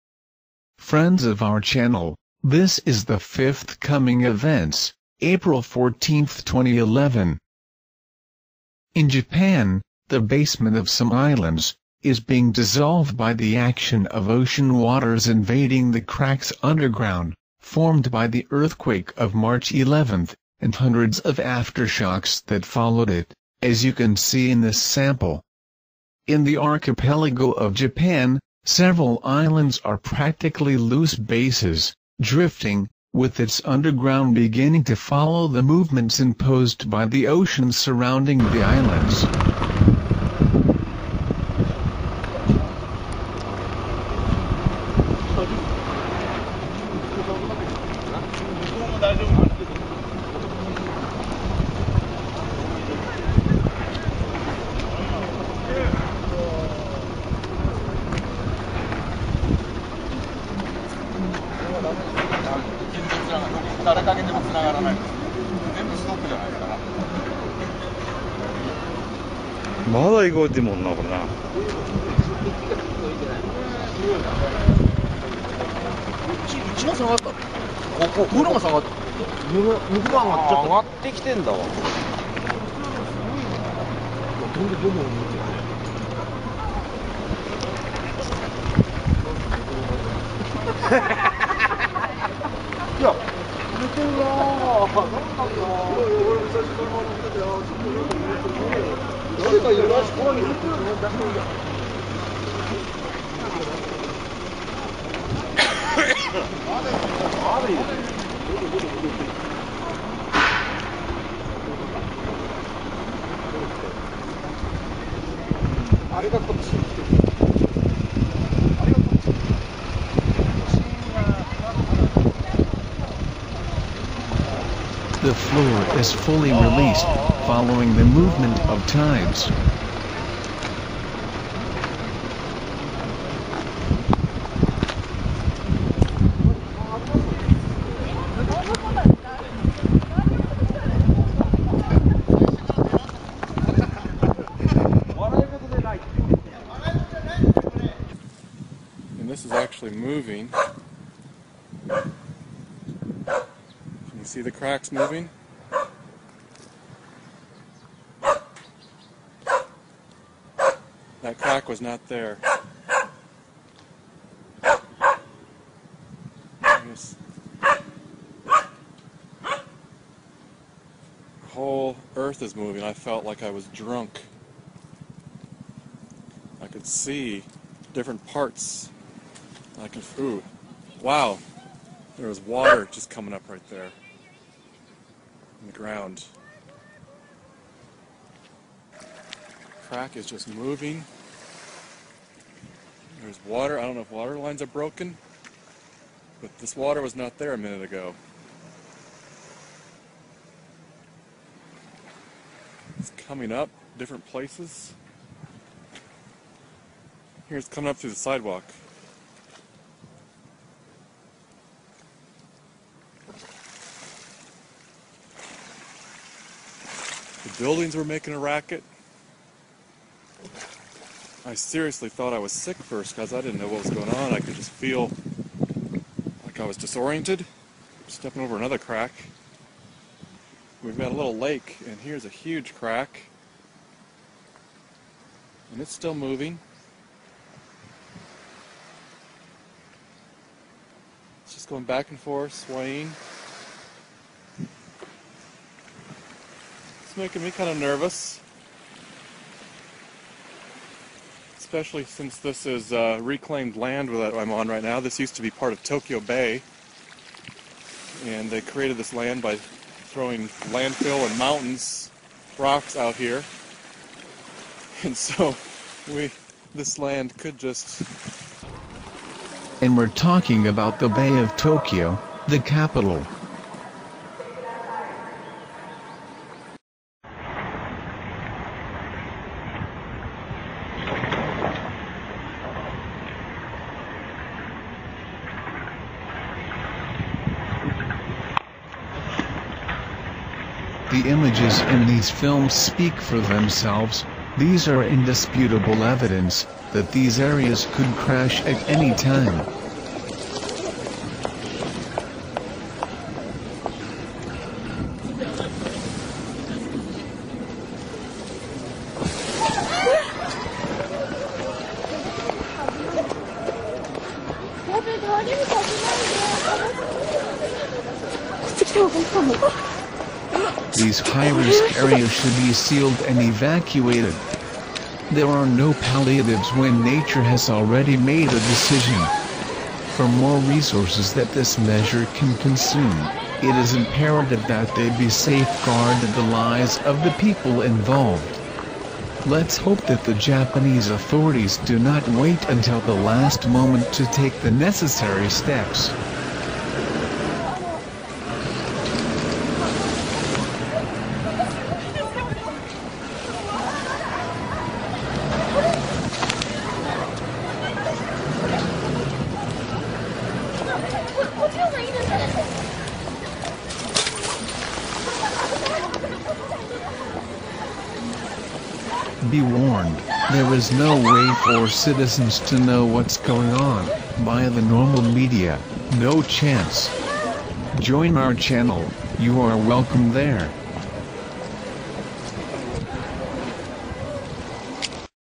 05 - NIBIRU - Next Events - Japan is sinking!
Friends of our channel, this is the fifth "coming events" signed by Captain Bill. April, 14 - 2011.

In Japan, the basement of some islands, is being dissolved by the action of ocean waters invading the cracks underground, formed by the earthquake of March 11, and hundreds of aftershocks that followed it, as you can see in this sample. In the archipelago of Japan, several islands are practically loose bases, drifting, with its underground beginning to follow the movements imposed by the ocean surrounding the islands. These high-risk areas should be sealed and evacuated. There are no palliatives when nature has already made ​​a decision. For more resources that this measure can consume, it is imperative that they be safeguarded the lives of the people involved. Let's hope that the Japanese authorities do not wait until the last moment to take the necessary steps.
The floor is fully released, following the movement of tides. And we're talking about the Bay of Tokyo, the capital.
Be warned, there is no way for citizens to know what's going on, by the normal media, no chance!

Captain Bill - April - 2011